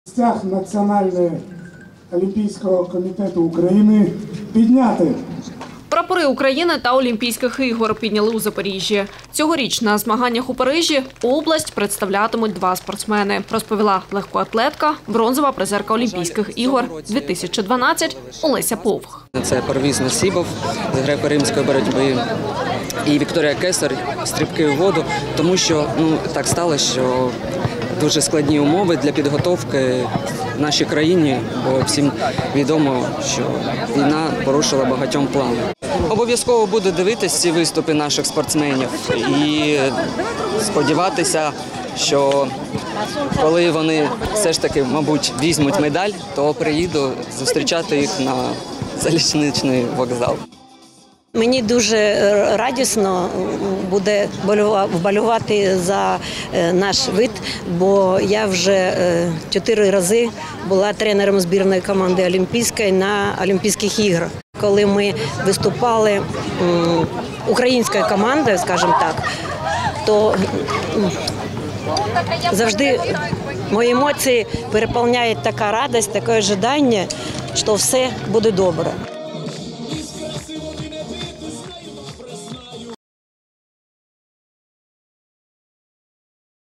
У Запоріжжі підняли прапори України та Нацкомітету напередодні відкриття Олімпіади-2024
Прапори України та Олімпійських ігор підняли у Запоріжжі. Цьогоріч на змаганнях у Парижі область представлятимуть два спортсмени — розповіла легкоатлетка — бронзова призерка Олімпійських ігор 2012 Олеся Повх.